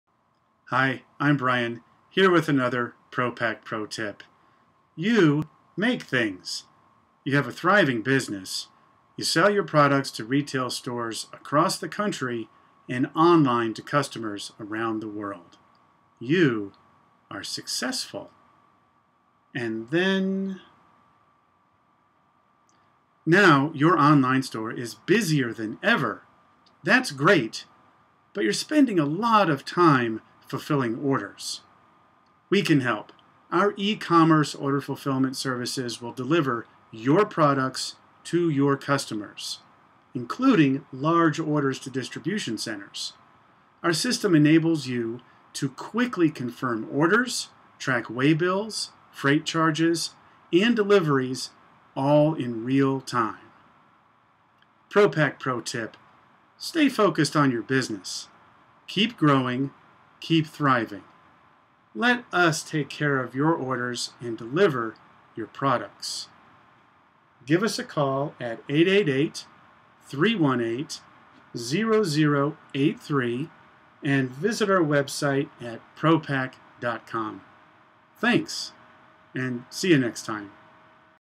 Pro Pac Pro Tip: Stay Focussed on Your Business, Let Us Take Care of Your Orders
You make things.  You have a thriving business.  You sell your products to retail stores across the country, and online to customers around the world.  You are successful.

Now your online store is busier than ever. That's great - but you are spending a lot of time fulfilling orders.

We can help.  Our ecommerce order fulfillment services will deliver your products to your customers.  Including large orders to distribution centers.  Our system enables you to quickly confirm orders, track waybills, freight charges and deliveries all in real-time.

Pro Pac Pro Tip: Stay focused on your business.  Keep growing.  Keep thriving.

Let us take care of your orders and deliver your products.